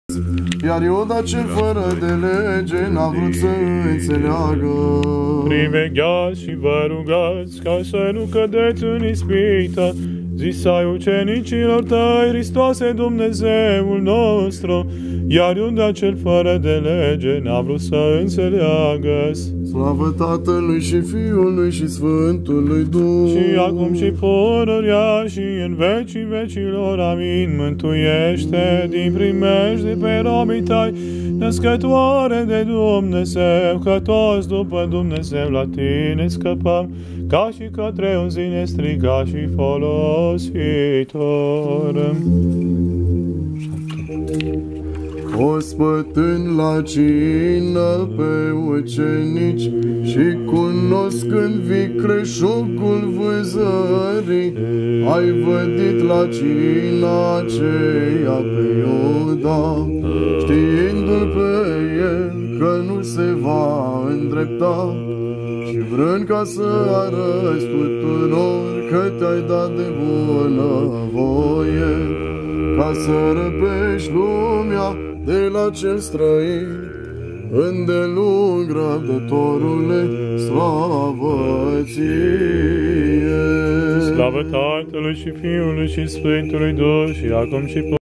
DENiA DE JOI SEARA LA Biserica Sf PAHOMiE din Bistrita anul 2020 PA.2#
A FOST TARE GREU DE PASTi ANU ASTA DE PANDEMiE !!! DIN CAUZA PACATELOR MELE SI ALE TALE CRESTINE ! HAi SA NE REVENiM !!!